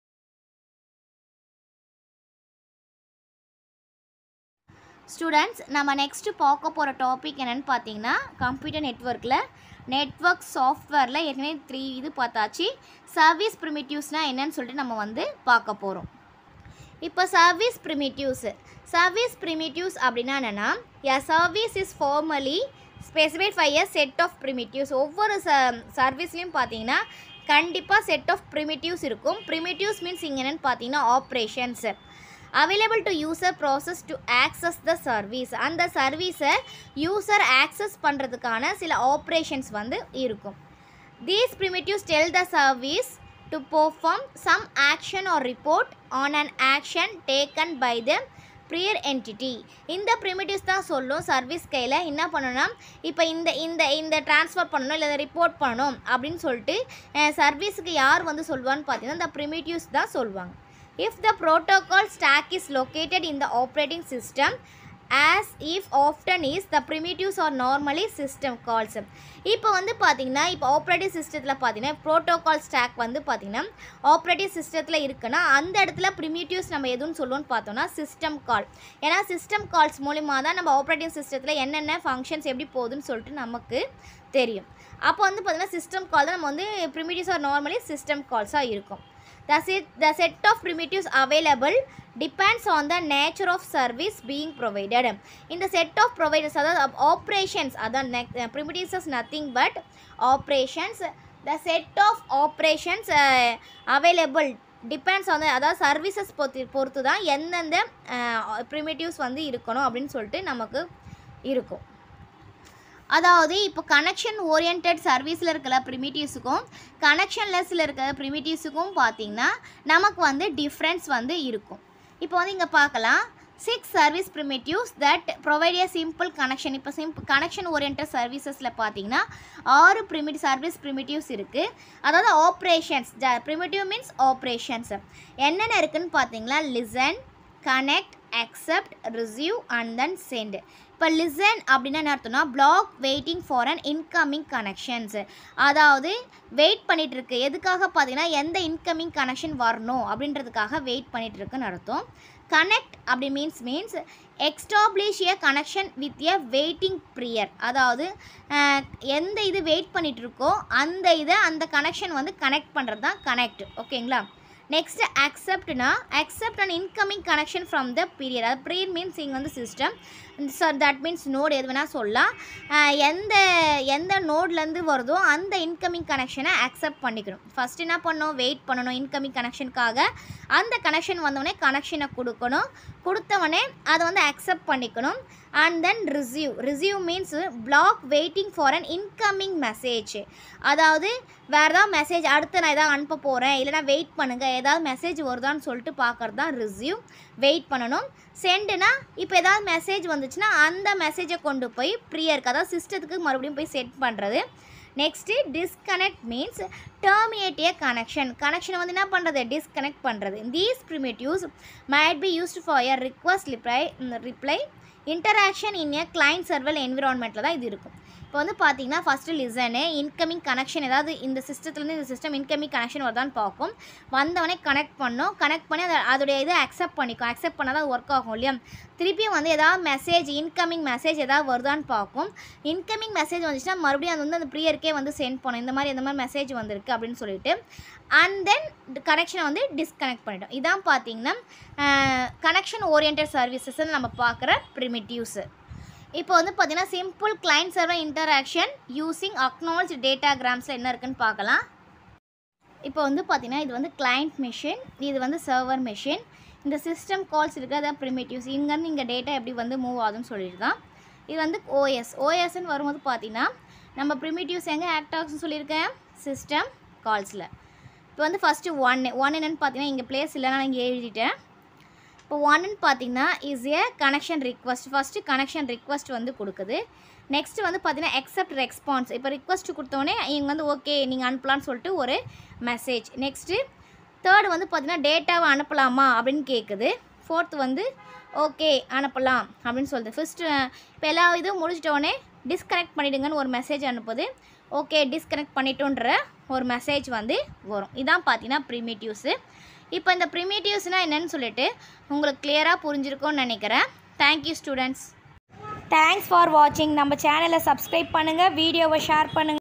Service Primitives | network software | computer networks in tamil
This content is about service Primitives in network software in computer networks in tamil with example